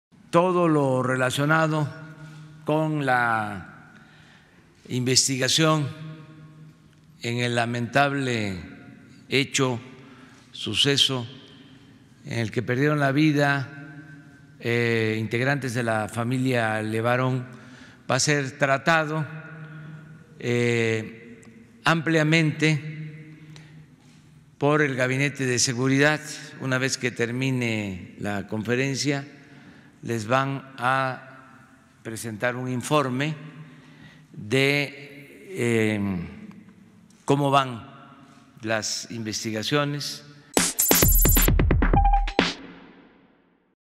Presentará gabinete de seguridad informe de investigaciones sobre ataque a familia LeBarón
El presidente Andrés Manuel López Obrador dijo que al terminar la conferencia matutina el Gabinete de seguridad presentará un informe respecto a las investigaciones llevadas a cabo por el ataque a la familia LeBarón